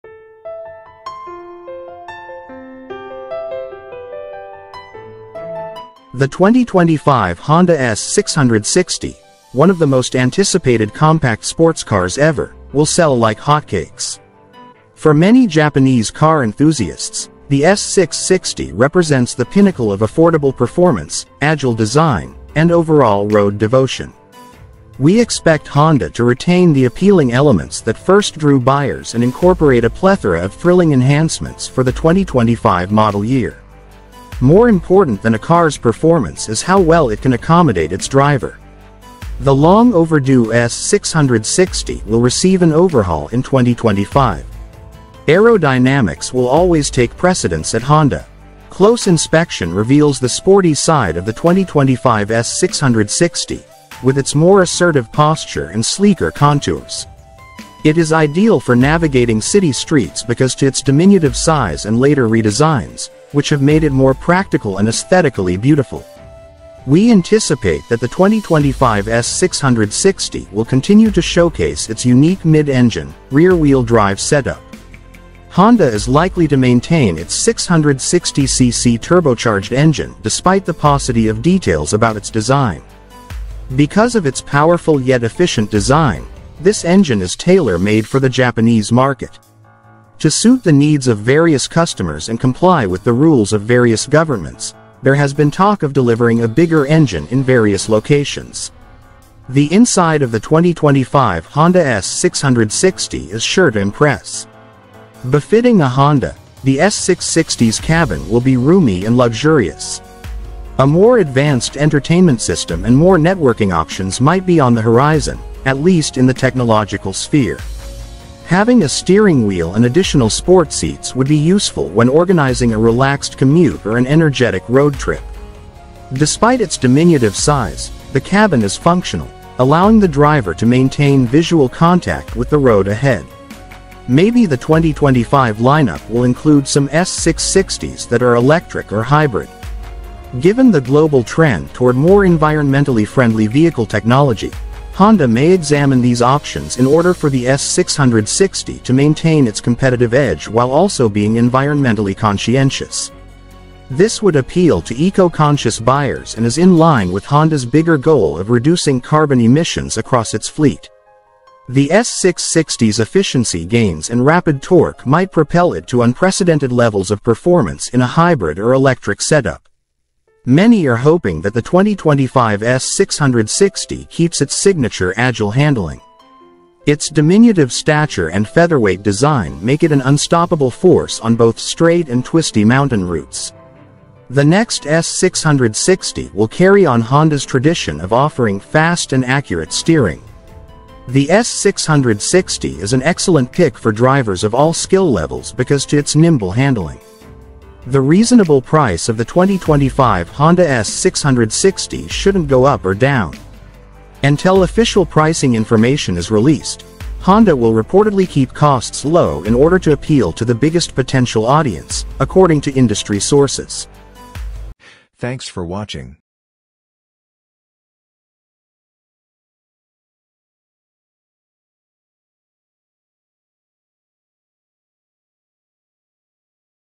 All New 2025 Honda S660 Unveiled - The Most Charming Small Sports Car ?
The 2025 Honda S660, one of the most anticipated compact sports cars ever, will sell like hotcakes. For many Japanese car enthusiasts, the S660 represents the pinnacle of affordable performance, agile design, and overall road devotion. We expect Honda to retain the appealing elements that first drew buyers and incorporate a plethora of thrilling enhancements for the 2025 model year. More important than a car's performance is how well it can accommodate its driver.

The long-overdue S660 will receive an overhaul in 2025. Aerodynamics will always take precedence at Honda. Close inspection reveals the sporty side of the 2025 S660, with its more assertive posture and sleeker contours. It is ideal for navigating city streets because to its diminutive size and later redesigns, which have made it more practical and aesthetically beautiful.

We anticipate that the 2025 S660 will continue to showcase its unique mid-engine, rear-wheel-drive setup. Honda is likely to maintain its 660cc turbocharged engine despite the paucity of details about its design. Because of its powerful yet efficient design, this engine is tailor-made for the Japanese market. To suit the needs of various customers and comply with the rules of various governments, there has been talk of delivering a bigger engine in various locations.